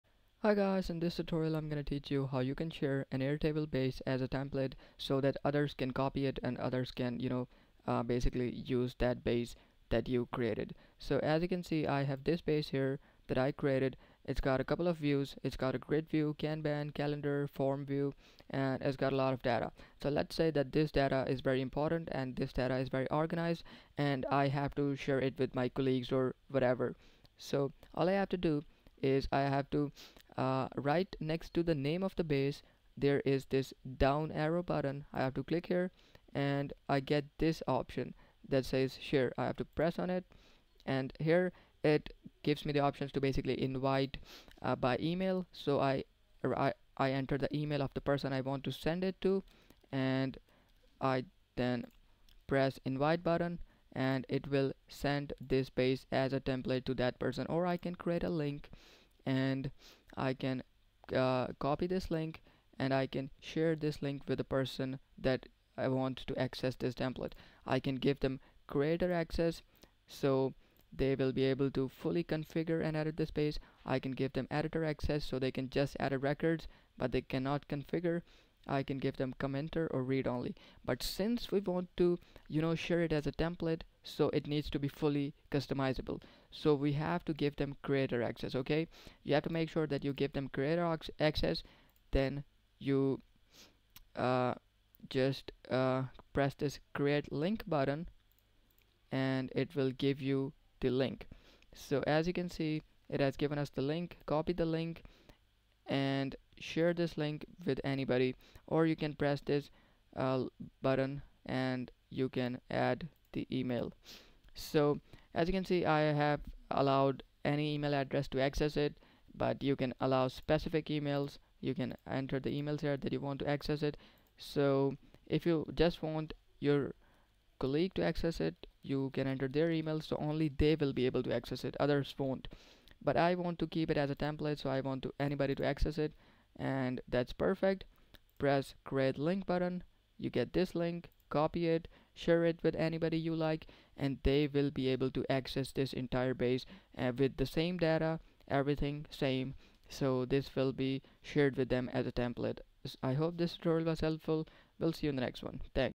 How to Share Airtable Base as a Template [Quick Guide]
Watch this short and simple tutorial to learn how to share a base as a template on Airtable.